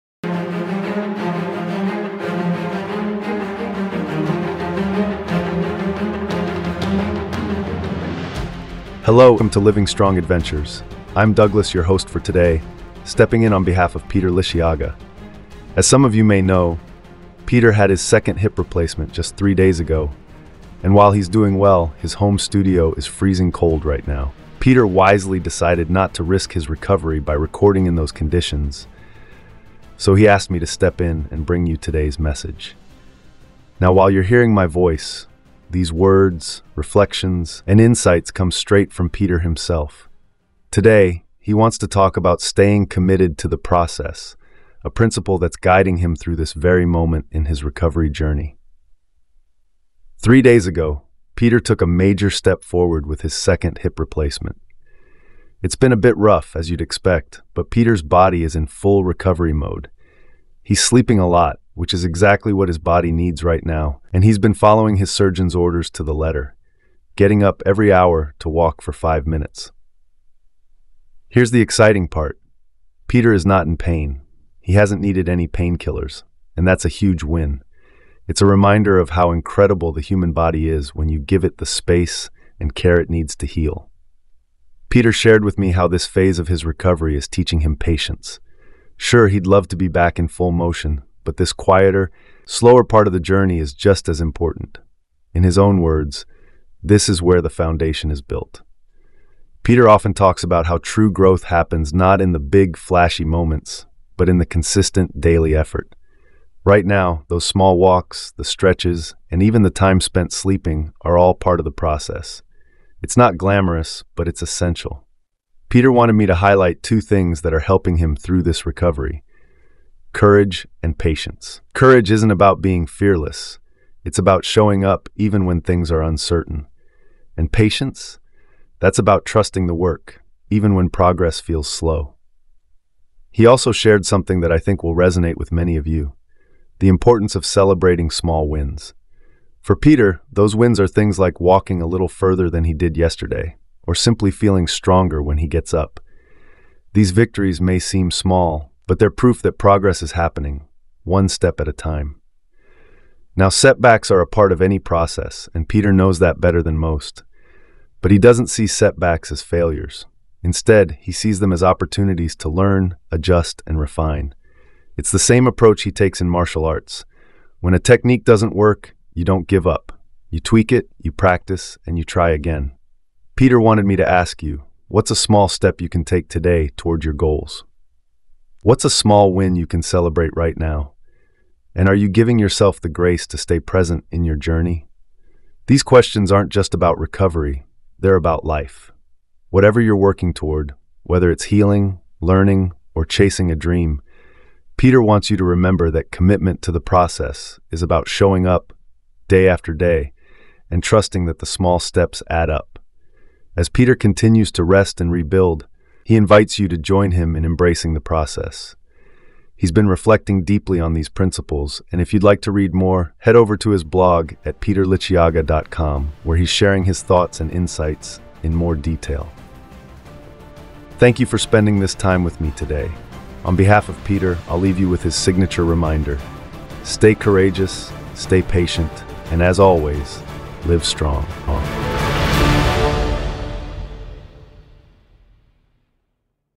Commitment to the Process: Trusting the Journey to Recovery
In this episode of Living Strong Adventures, Gregory hosts on behalf of Peter Liciaga, who is three days into recovering from his second hip replacement. Peter’s journey is a testament to the power of staying committed to the process, even when progress feels slow.

Hear Peter’s reflections on the importance of courage, patience, and celebrating small victories—principles that guide him not only through recovery but in life and martial arts. This heartfelt conversation will inspire you to trust your own journey and find strength in the small, deliberate steps forward.

Join us for a powerful reminder that the process is where transformation happens.